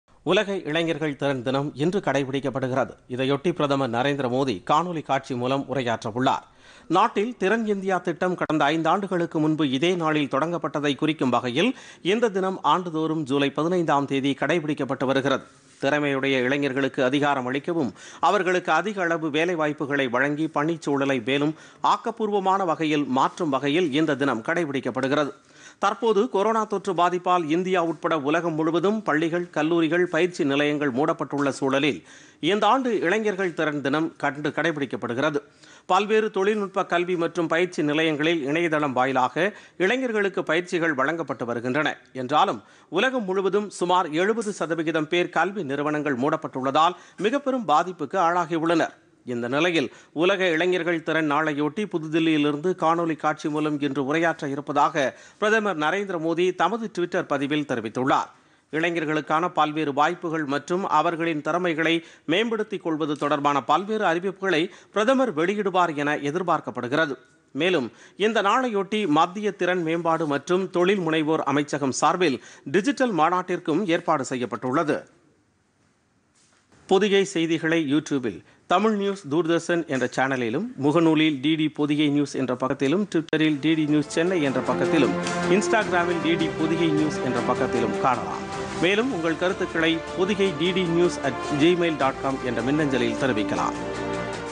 உலக இளைஞர்கள் திறன் தினம்
உலக இளைஞர்கள் திறன் தினம், இன்று கடைபிடிப்பு: பிரதமர் நரேந்திர மோதி காணொலி காட்சி மூலம் உரையாற்றுகிறார்.